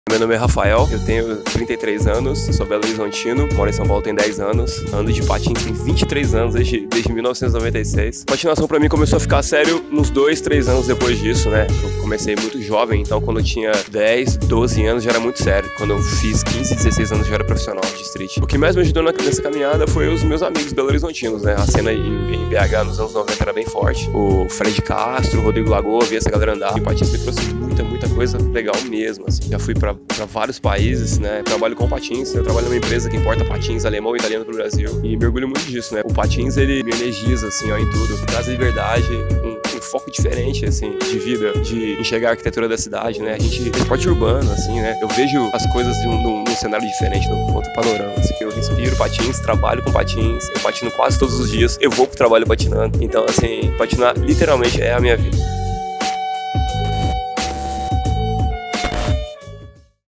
Treze Core Lifestyle #32 - Patinador Raphael Lino
O patinador Raphael Lino participou do Treze Core Lifestyle: projeto que tem como foco divulgar pessoas de diferentes estilos de vida que representam nossa marca.

Compartilhe e de aquele like pra valorizar o trabalho!

Patinador: @raphalino23
Vídeo: @izabelacardozo

CONHEÇA NOSSA MARCA!